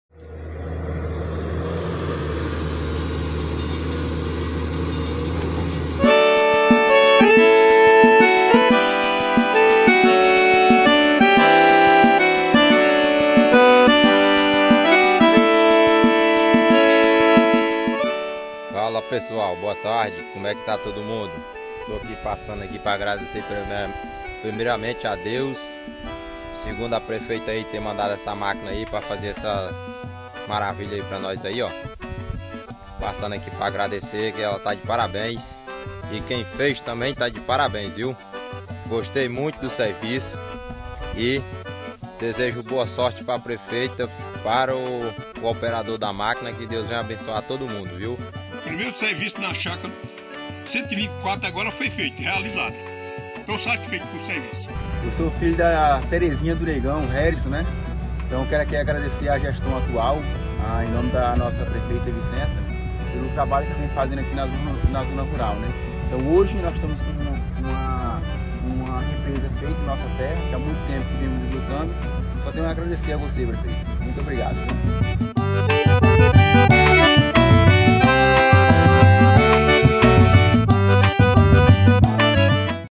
Represas Novas na Dalila
Benefício ao pequeno produtor. Parabéns Prefeita Vicença Lino, Parabéns Deputado Federal Tiago Dimas .